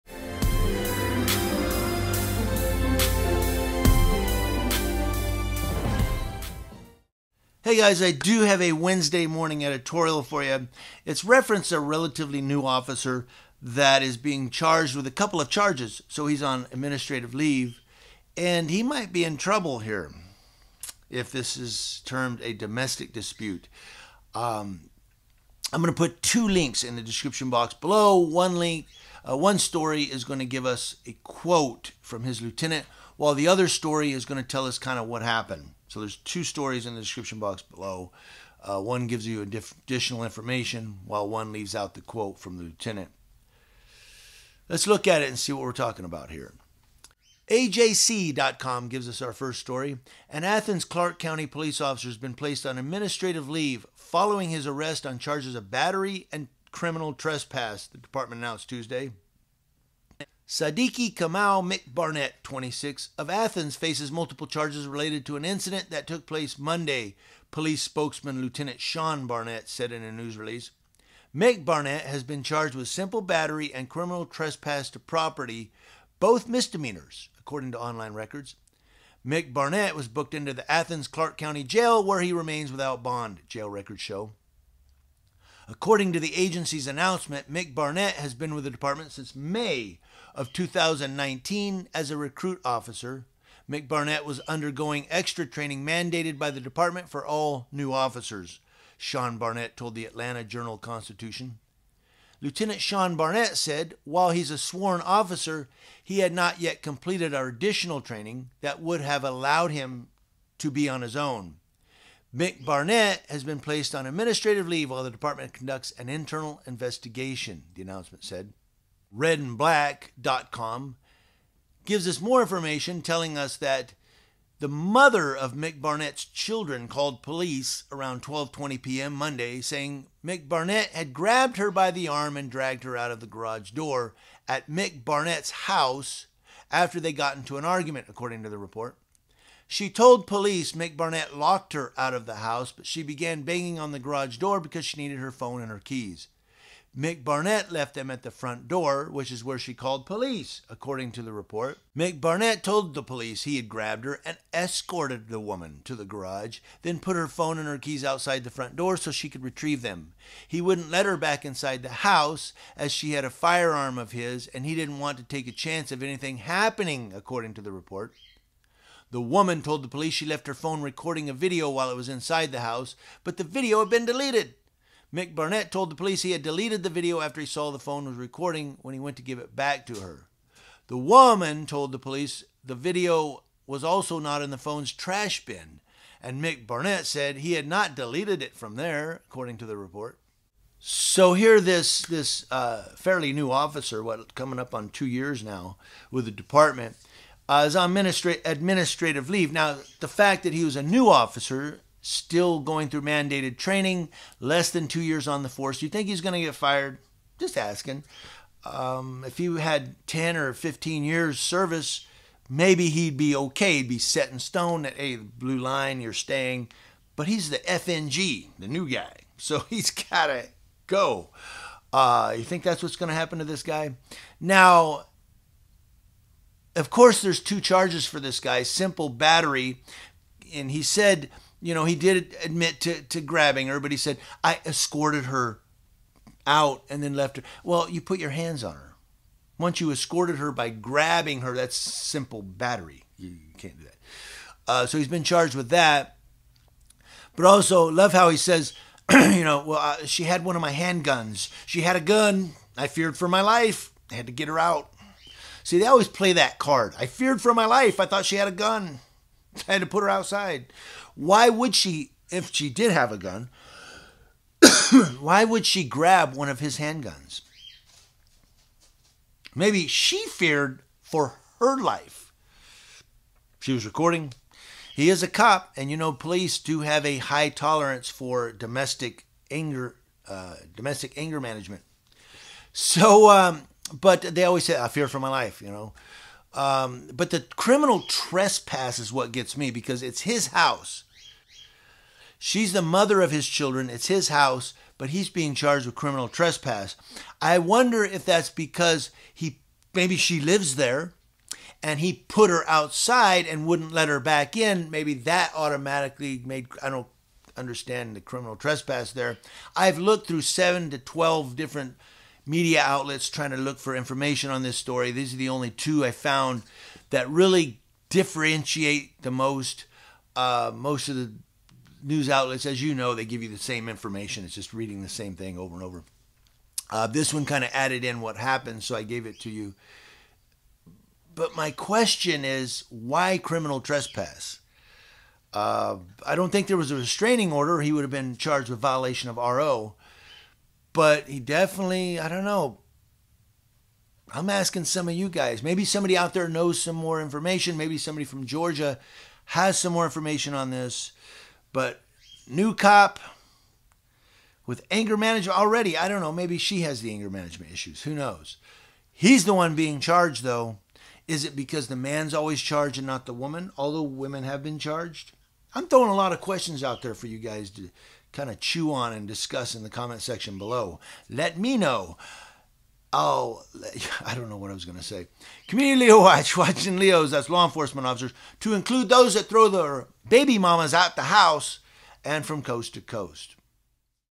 Police Officer in Athens Charged in Battery
Athens Cop arrested